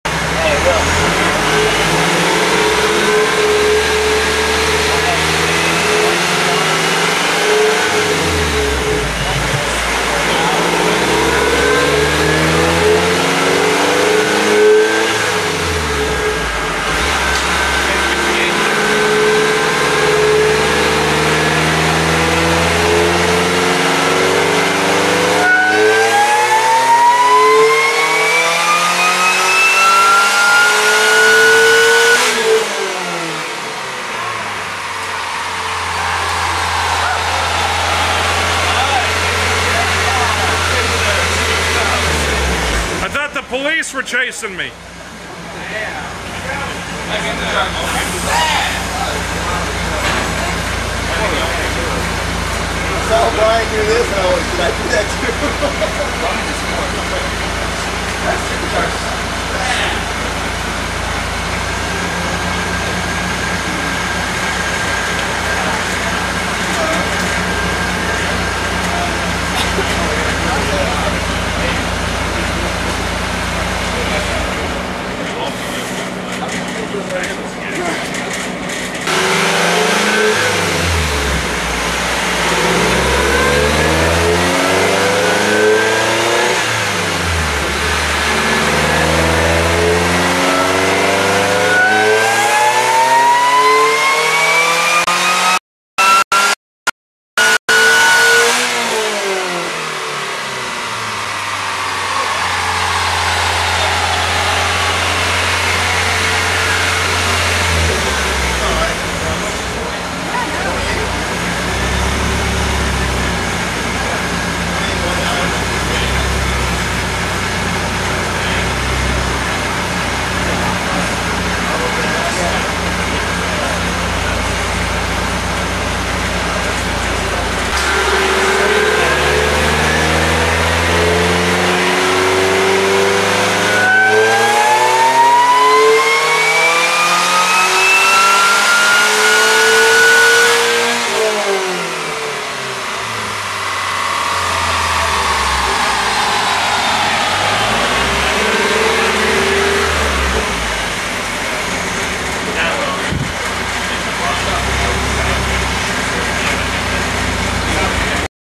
EAG Dyno Day - Jason's SC'd Solstice
EAG Dyno day at Car Boy
Jason's supercharged Solstice...